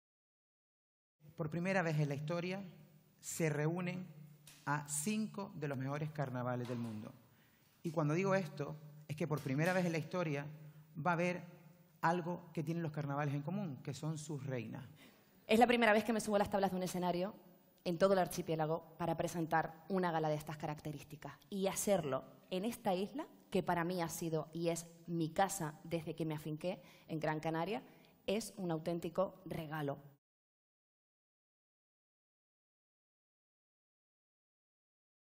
Presentación de la Gala de la Reina del Carnaval de Las Palmas de Gran Canaria
La Gala de elección de la Reina del Carnaval de Las Palmas de Gran Canaria ha dado a conocer algunas de las sorpresas que gurda. 5 reinas de los mejores carnavales del mundo estarán presentes.

Será conducida por Dani Calero y Wendy Fuentes, presentadores de Televisión Canaria. Josué Quevedo ha destacado que será una gala "como las de siempre, pero con mucho más ritmo"

Intervienen:
Josué Quevedo, director del carnaval de Las Palmas de Gran Canaria
Wendy Fuentes, presentadora de la gala